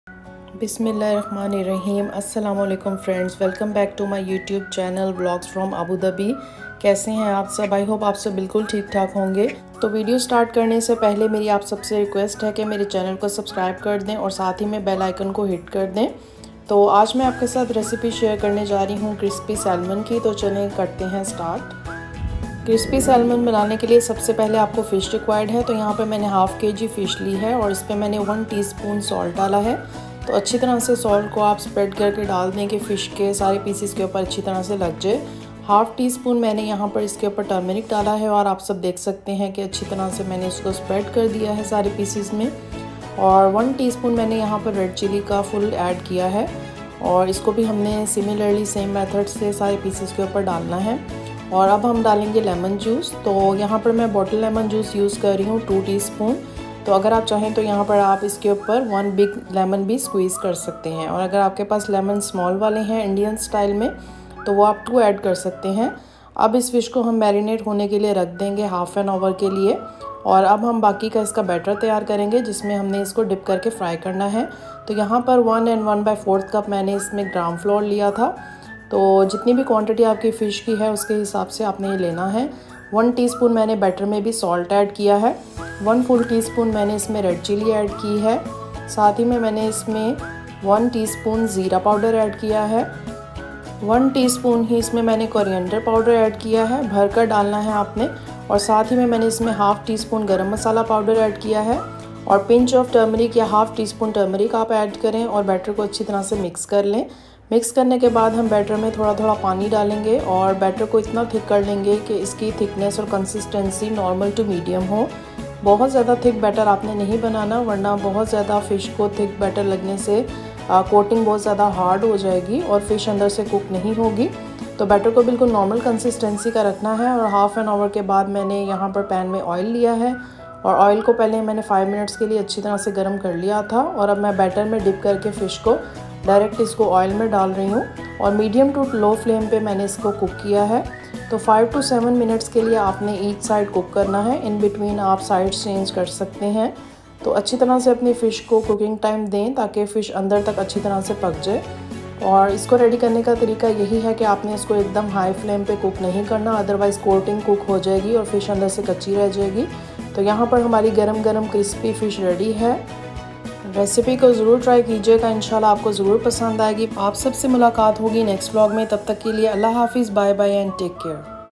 Crispy Salmon|How To Cook Crispy Fried Salmon|Lahori Style Fried Fish Recipe
Fried fish with coating..